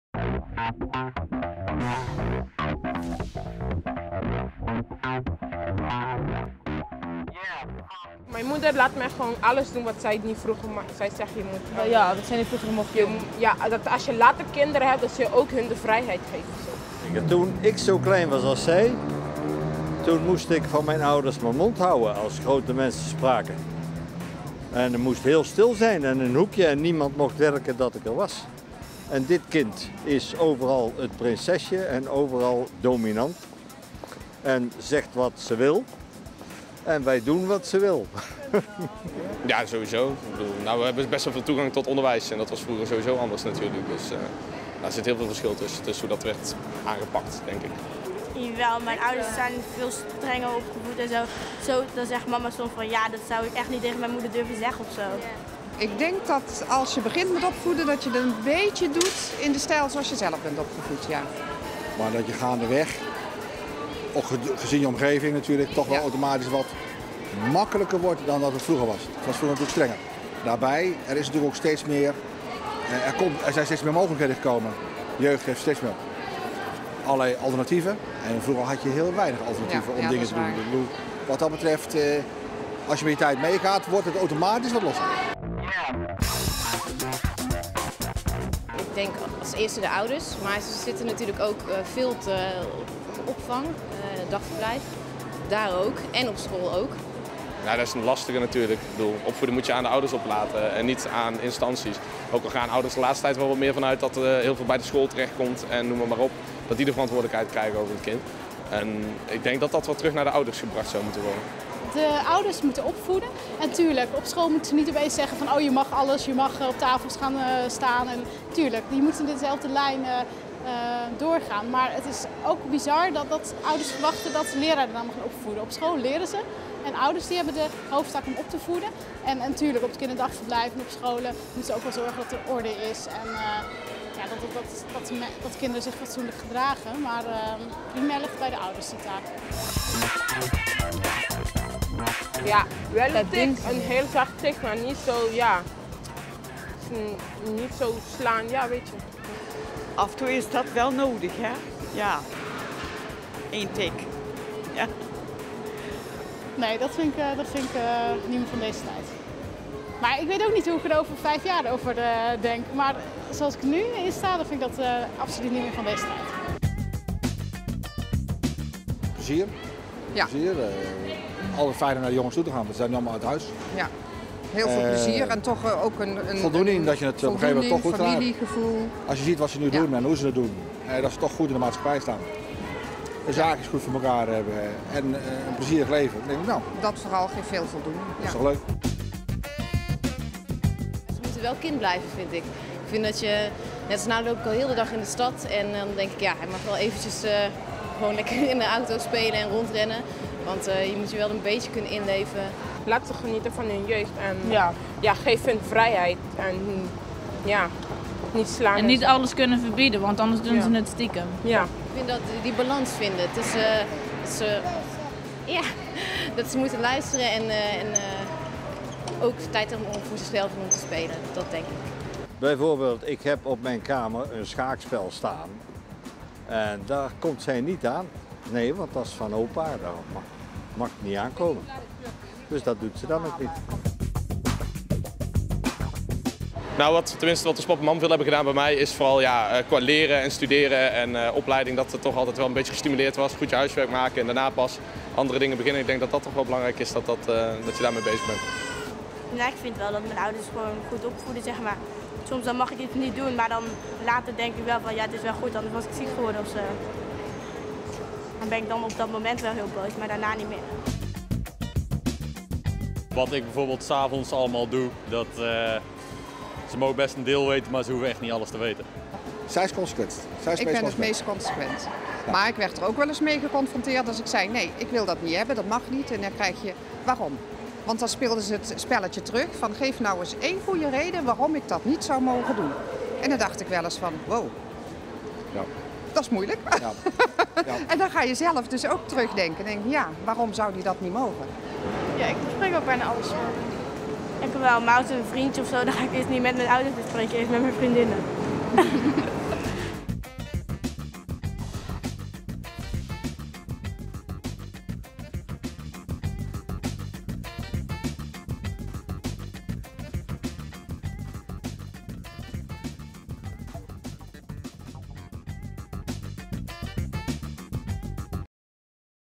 Interviews jeugd den Bosch
Passanten in Den Bosch worden geïnterviewd over jeugdzaken.